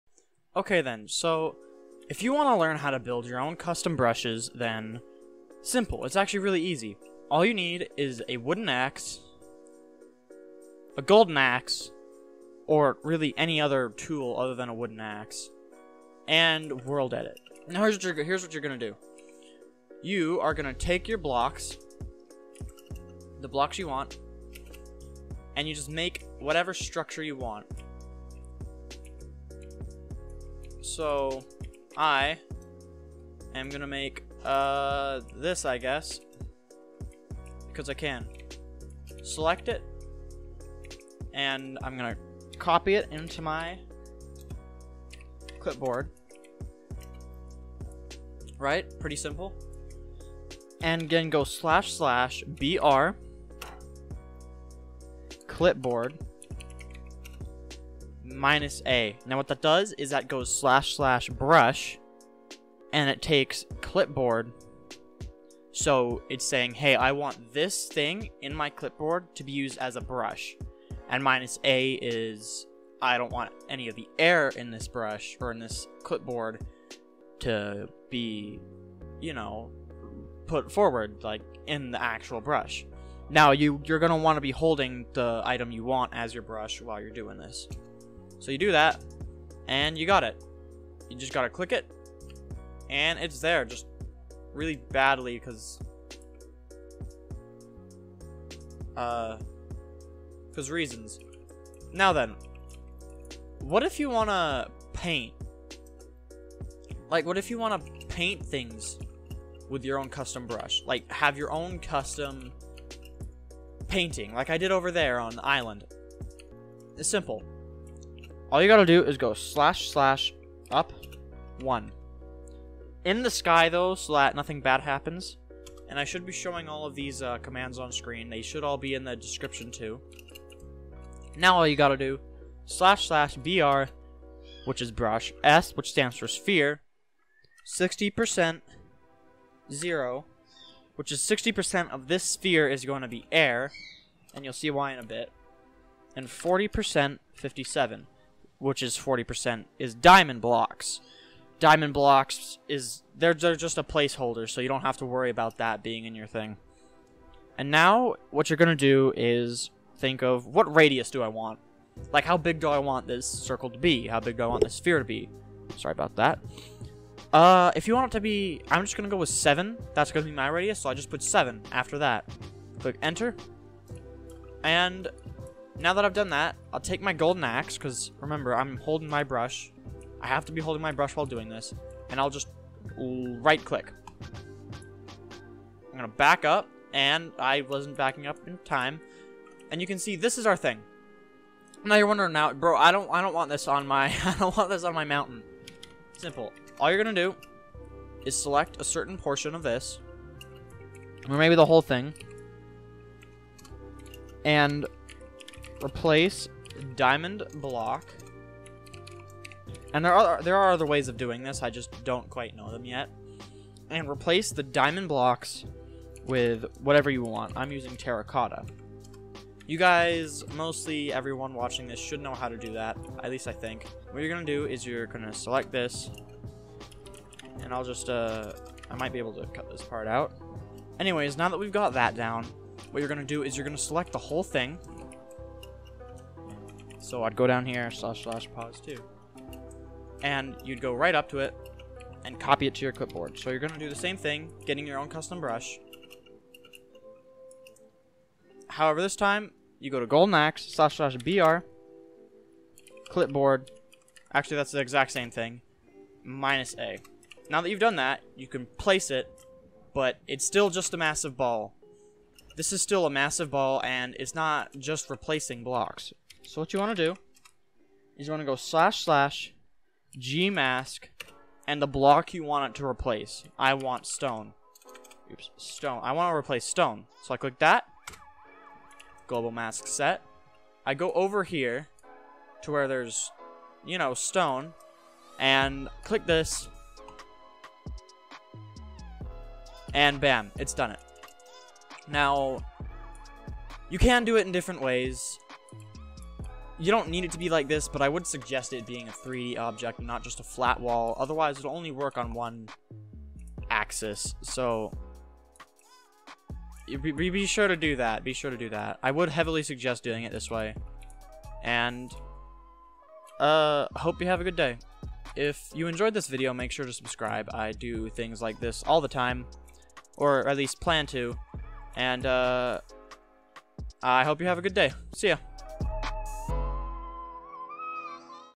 how to make your own custom brush with world edit!
Today we're learning how to make custom brushes with world edit!

this is actually a really simple thing to do and expect almost everyone to nail it first or second try!

commands:
(sorry will add later but I'm doing this in a hurry right now so sorry)

I hope this helps and have a good day!